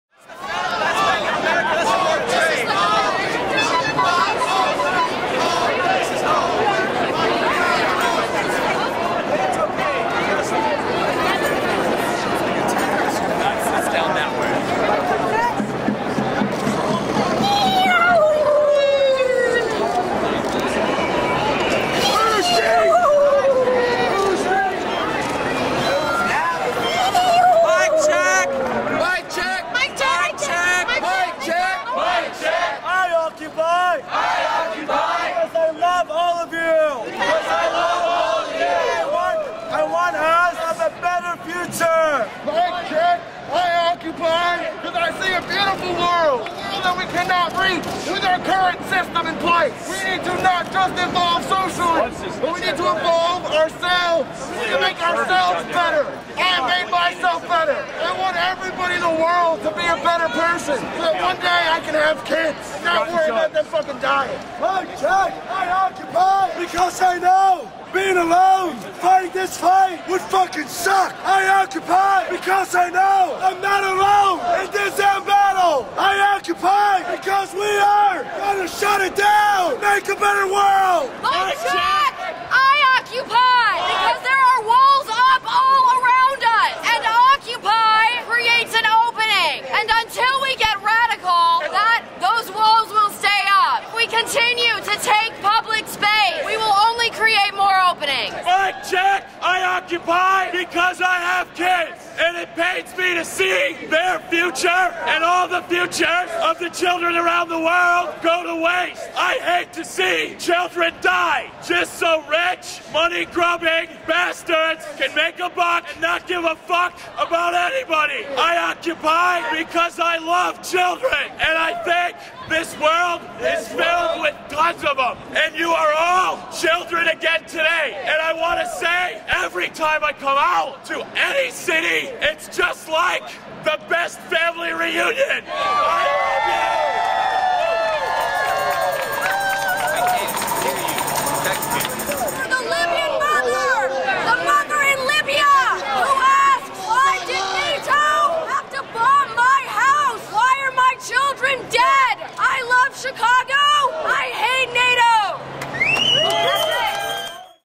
Why I Occupy: Chicago 18 May 2012
We're in Chicago, covering the anti-NATO protests. I shot this during the the first demonstration we covered and editing it the same day.